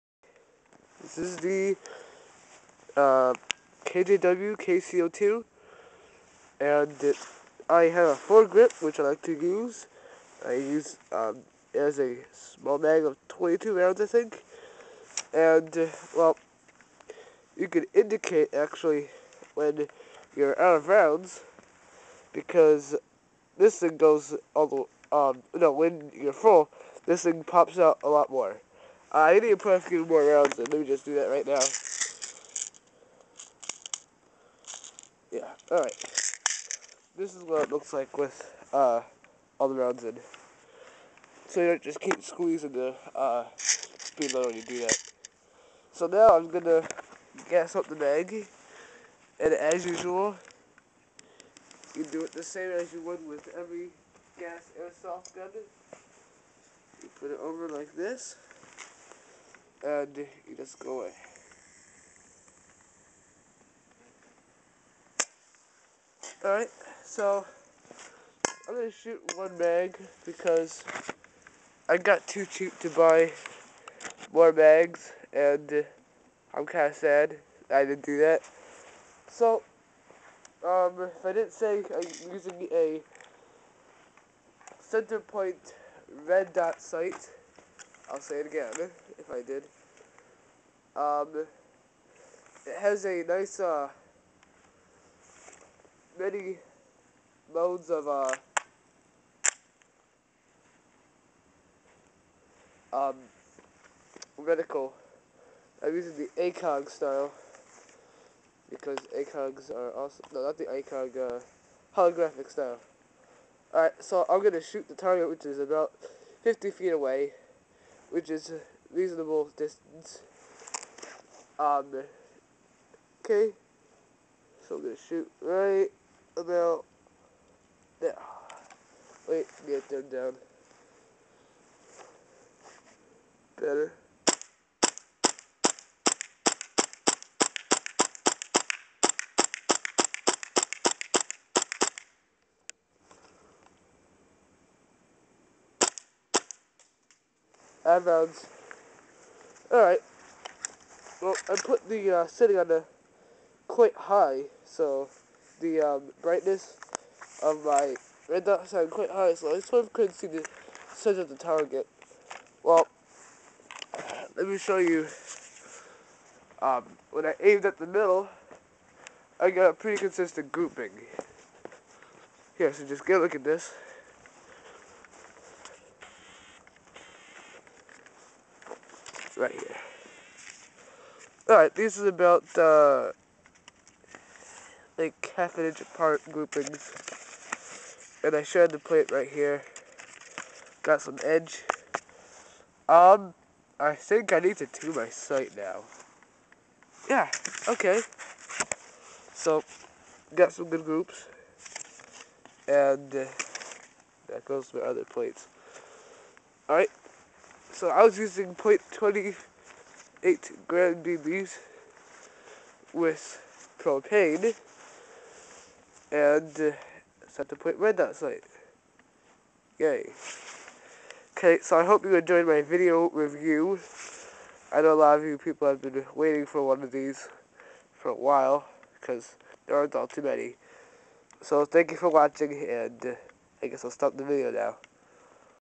Kjw kc02 airsoft review
This is the kjw kc02. It is a very good gun, great distance, around 200 ft. good for cqb "snipng" which s 170 ft sniping with "trigger finger" subscribe, I put the video out because there aren't many video reviews on this gun.